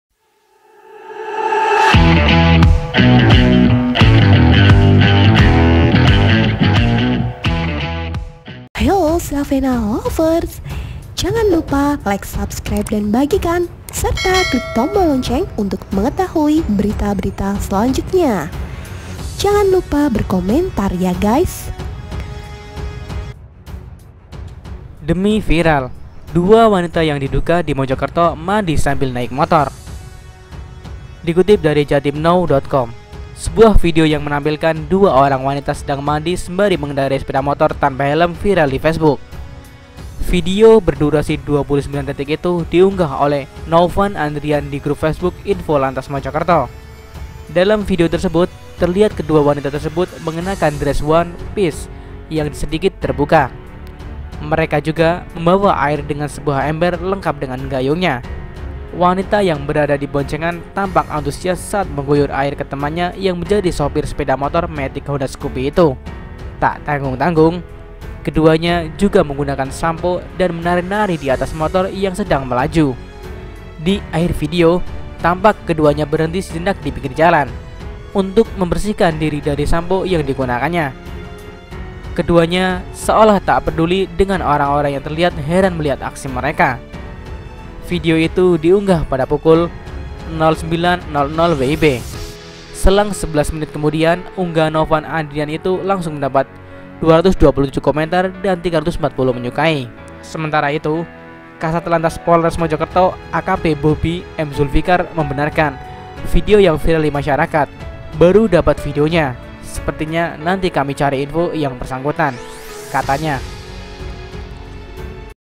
Demi Viral !!! 2 Wanita yang Diduga di Mojokerto Mandi Sambil Naik Motor
MandiDiAtasMotor Terima kasih sudah berkunjung ke Channel Zavena, Portal gosip yang paling up to date.